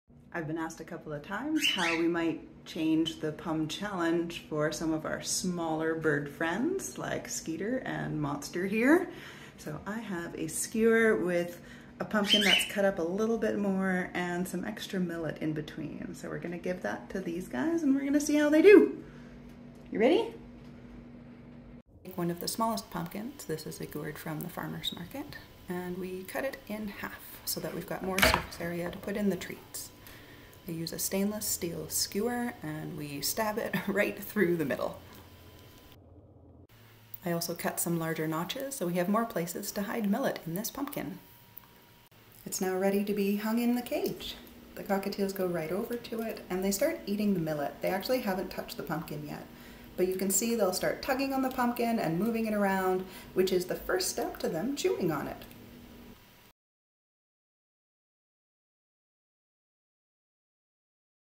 Pumm Challenge Smal Bird
This is how I get the smaller birds interested in chewing on a pumpkin for the PumpkinsForParrots